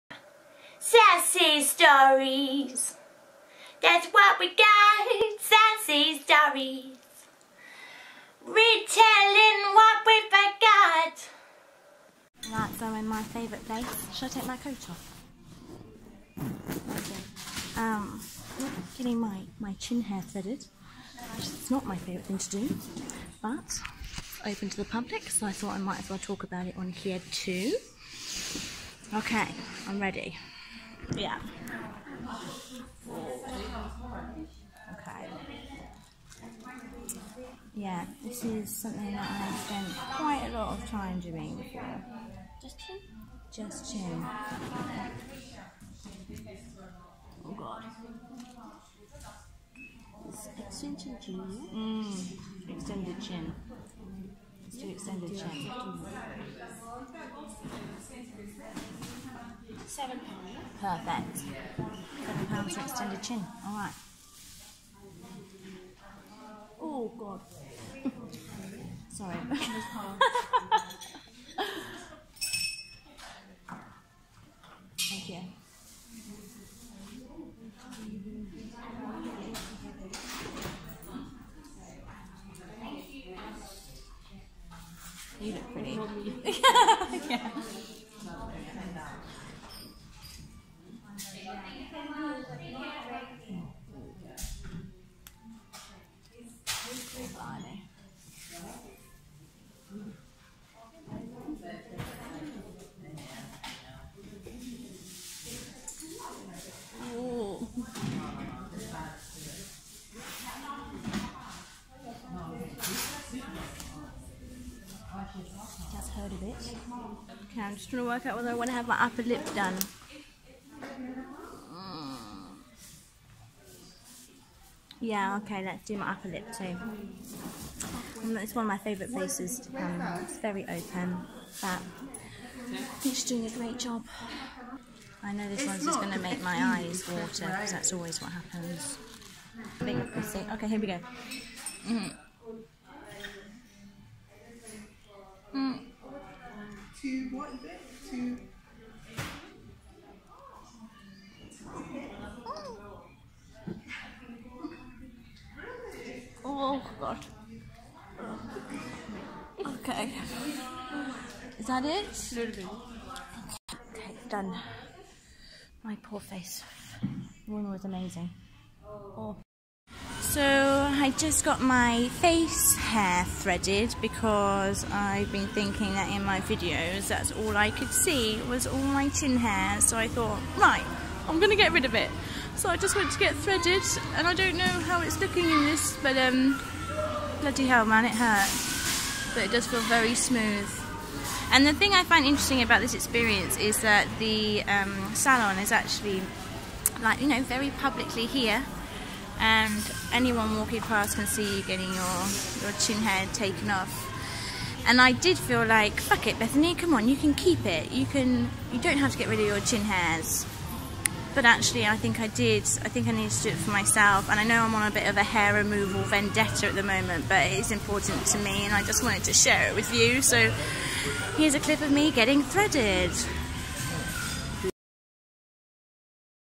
THE SASSY SHOW - EPISODE 3: BETHANY GETS HER CHIN HAIR THREADED
THE SASSY SHOW is an extension of Sassy Stories Blog

Bethany Burgoyne shares her trip to a beauty salon situated in the middle of a shopping center in East London. We see Bethany get her chin and upper lip hair threaded, highlighting some of her own insecurities and the lengths she goes to to feel better about her face.

Bx Sassy